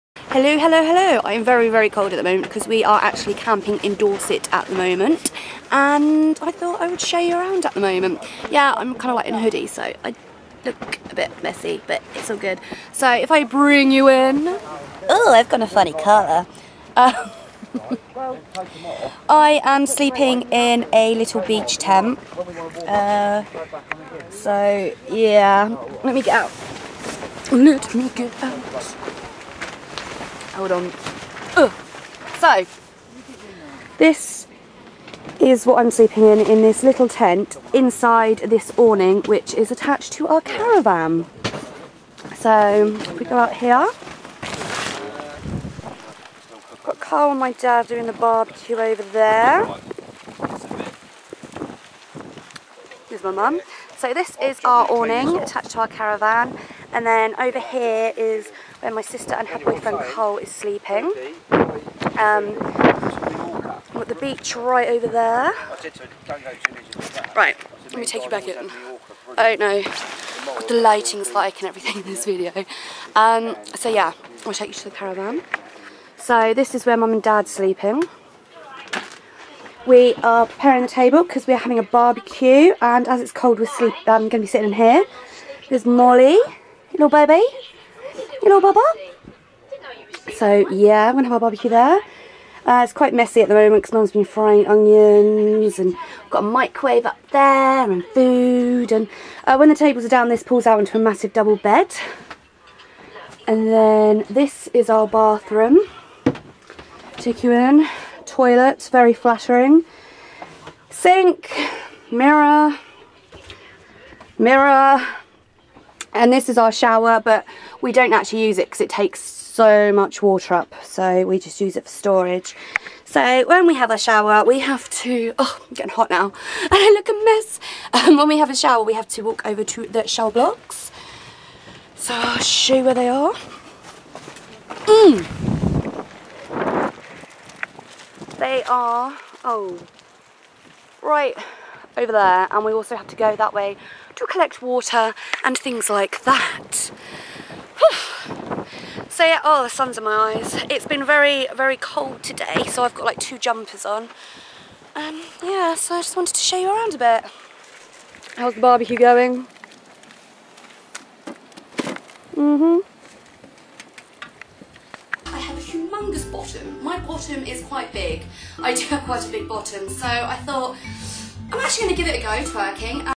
camping and caravan tour! ~Dorset - fresh water
come check out my camping and caravan tour! staying at freshwater, burton bradstock in dorest.